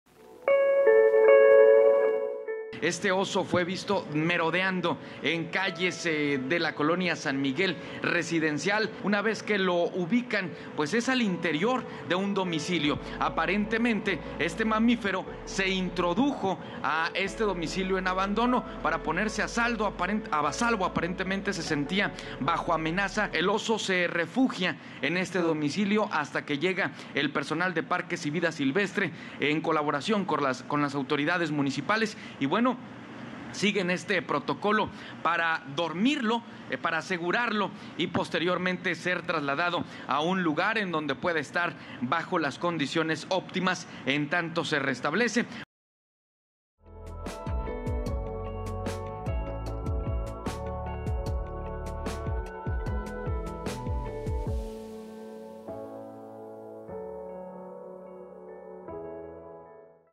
Salvan a oso de golpe de calor en Escobedo, Nuevo León - N+
Autoridades lograron rescatar a un oso de un golpe de calor en la colonia San Miguel residencial en Escobedo, Nuevo León.